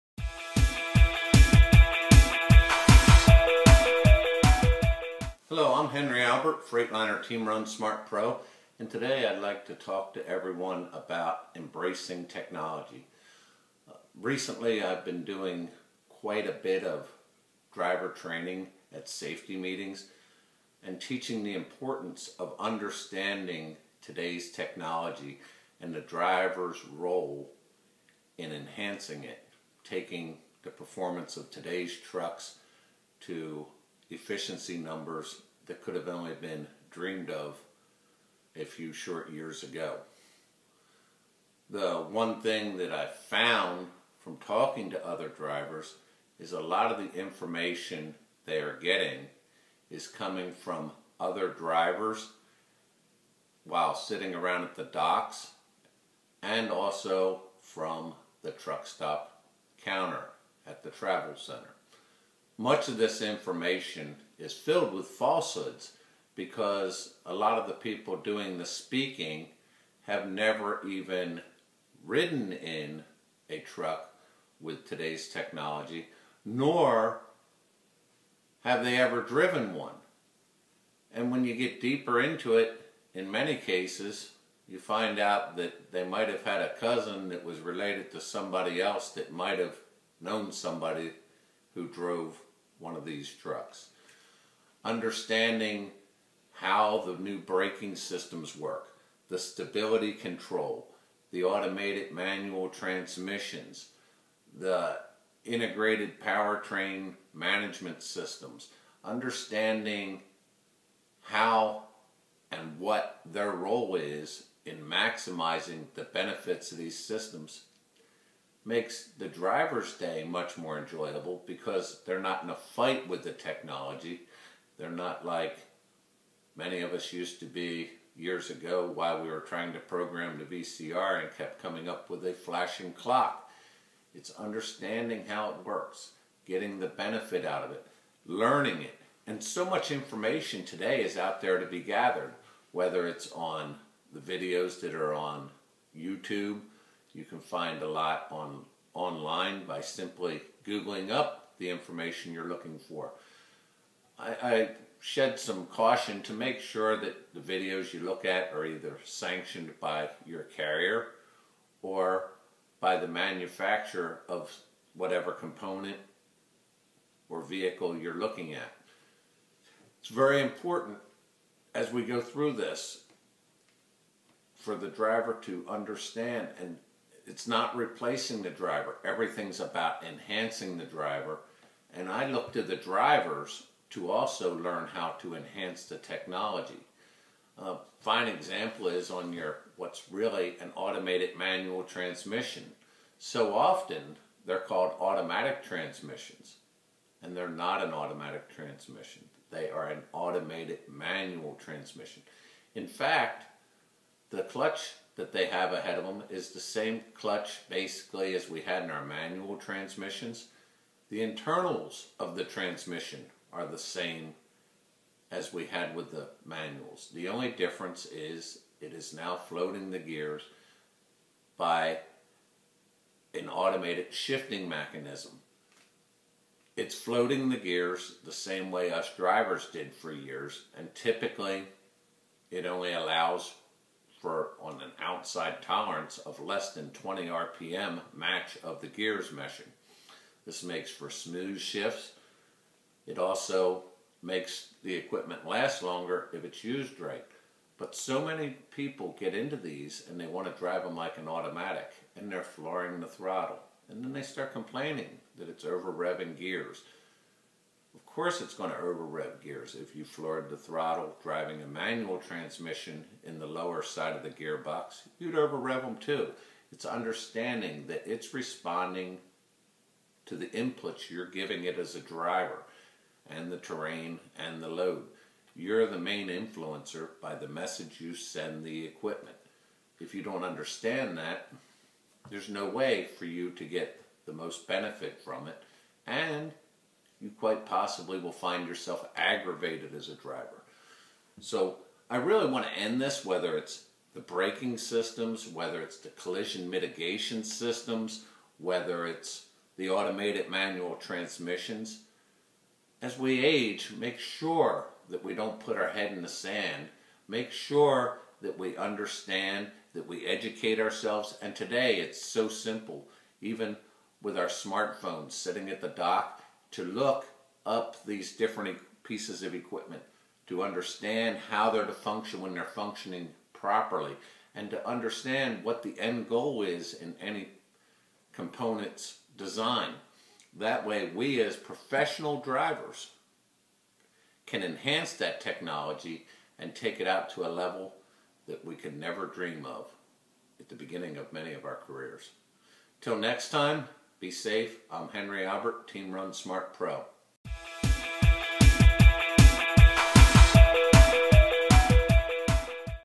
Embracing Technology
Team Run Smart Pro Henry Albert talks about the importance of embracing technology in your truck.